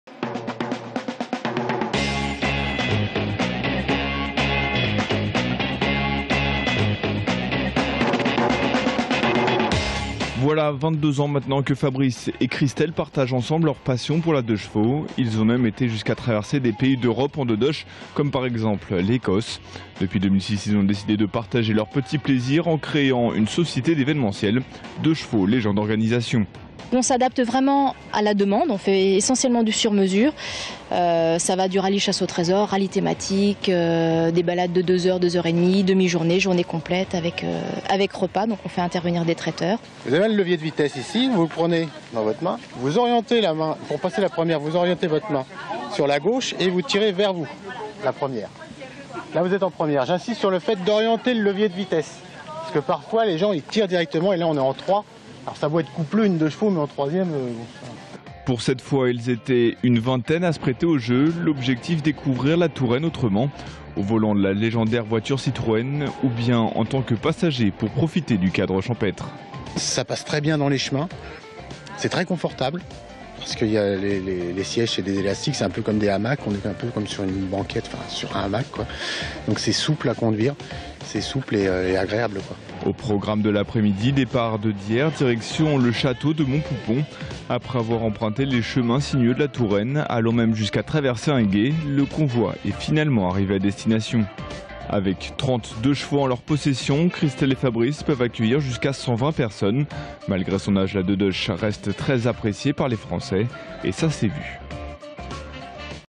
La deudeuche reine du rallye touristique
Sites webs/blogs, copiez cette vidéo avec le bouton "Intégrer" !
 Portrait de Christèle et Fabrice Legendre, passionnés de 2CV.